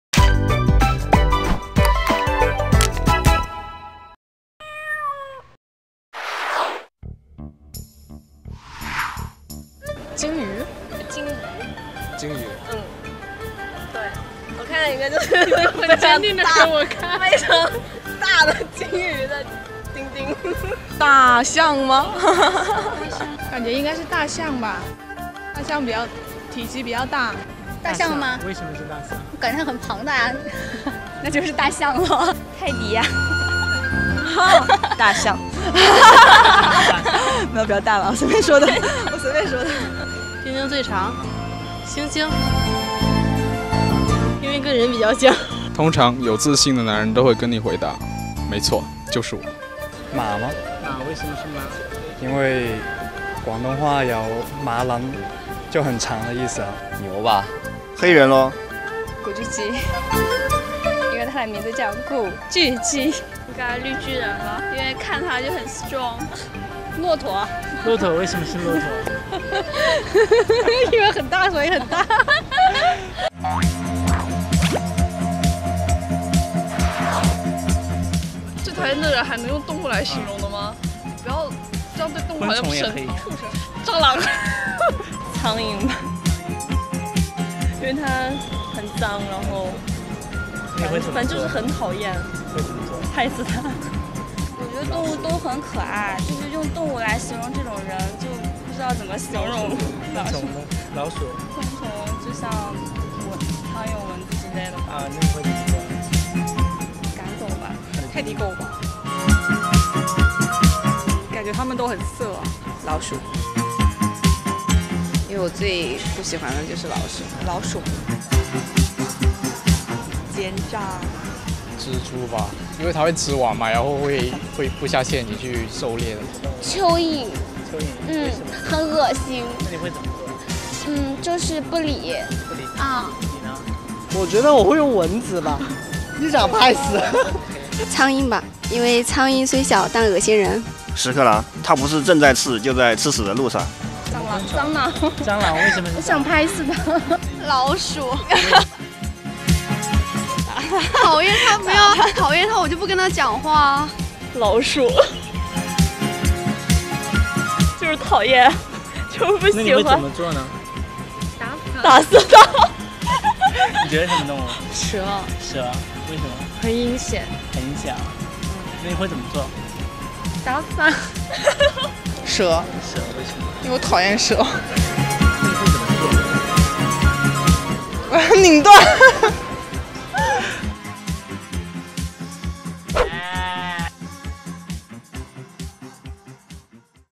什么动物丁丁最长？
“喵了个汪”第五期街访视频，什么动物丁丁最长？用一种动物形容你讨厌的人